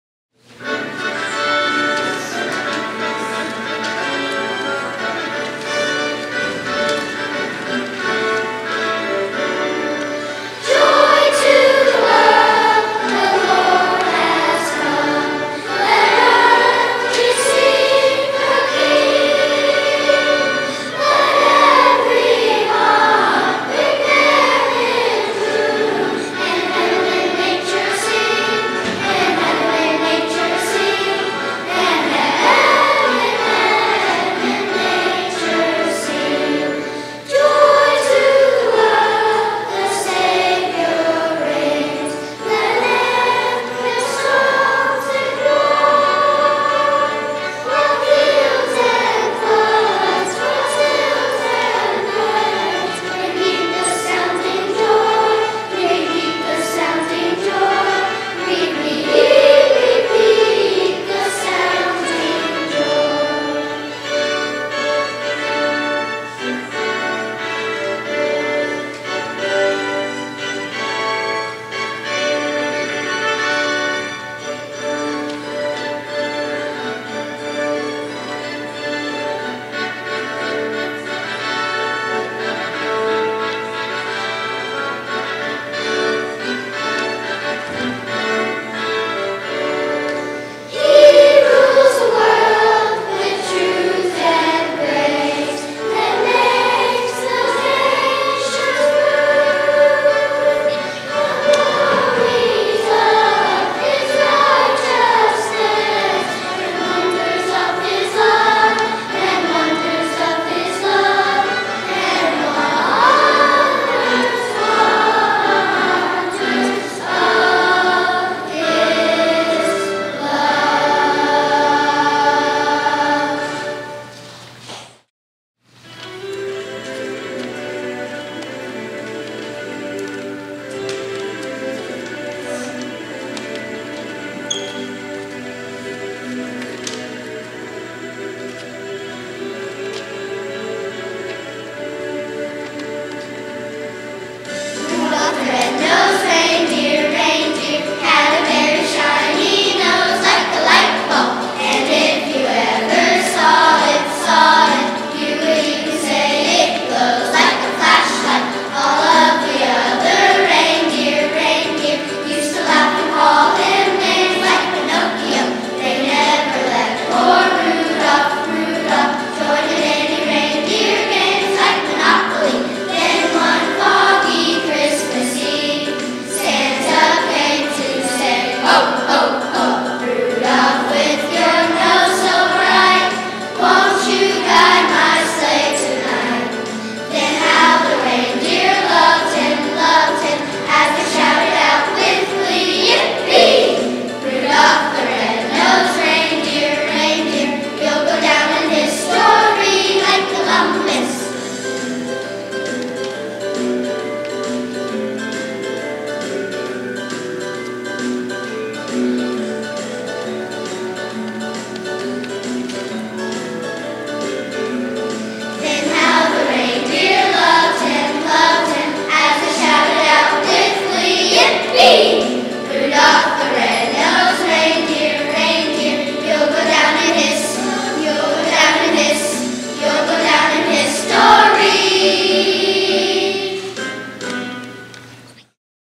St. Paul's Episcopal 2015 - Sounds of the Season 2
St. Paul's Episcopal joins Sounds of the Season at City Hall in Waco.